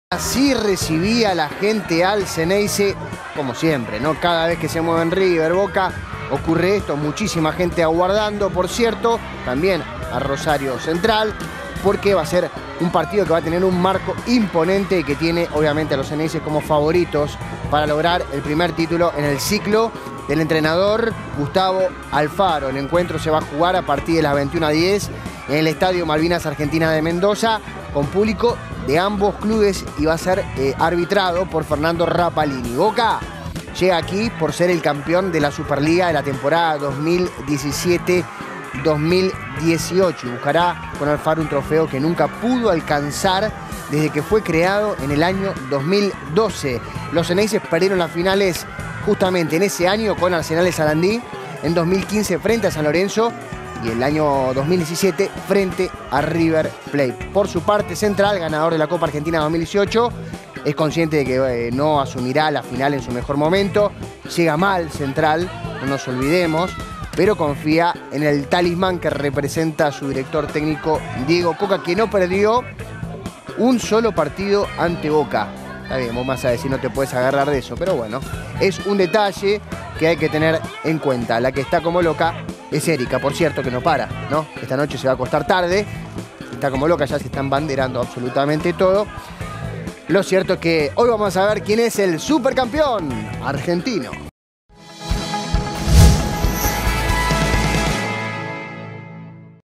Esta noche Boca y Rosario Central definen al campeón de la Supercopa Argentina
Se enfrentarán desde las 21.10, en Mendoza.